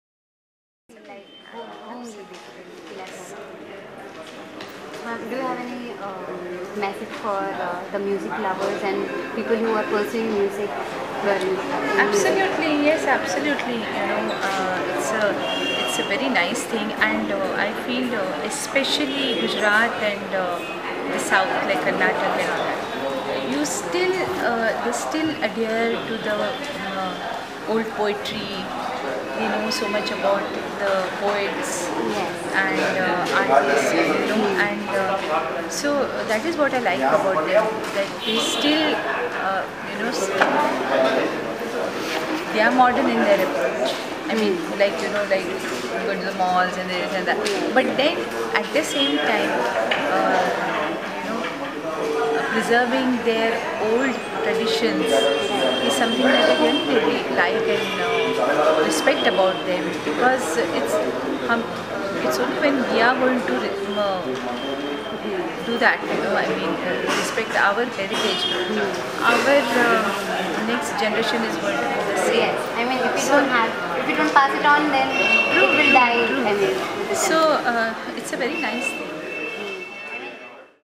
SPH One Minute Stories - Anuradha Paudwal Part 2
In this Part 2 of the talk, watch Anuradha give advise to music lovers and passionate upcoming musicians of Baroda.

Surya Palace 1 minute stories are short videos that we would like to share with all our guest, patrons and friends on social media.